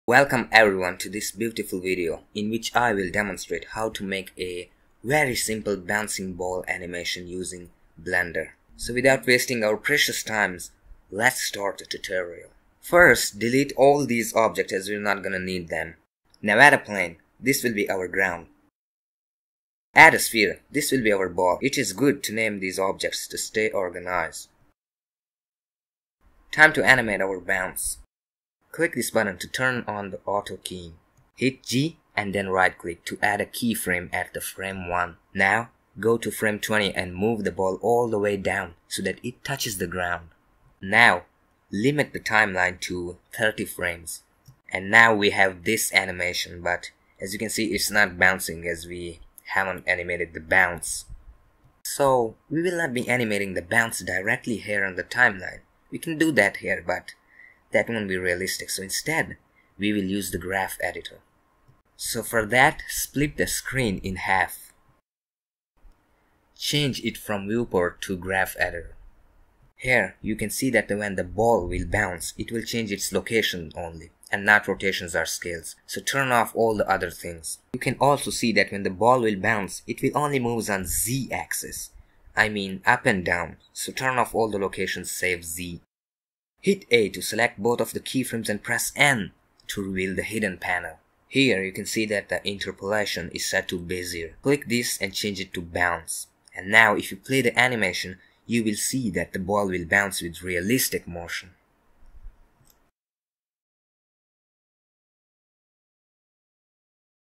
How To Make A Bouncing Ball Animation In Blender 3.5
Learn making a ball bounce very easily in Blender 2.9. After watching this video, You can do it easily even if you are completely new to Blender!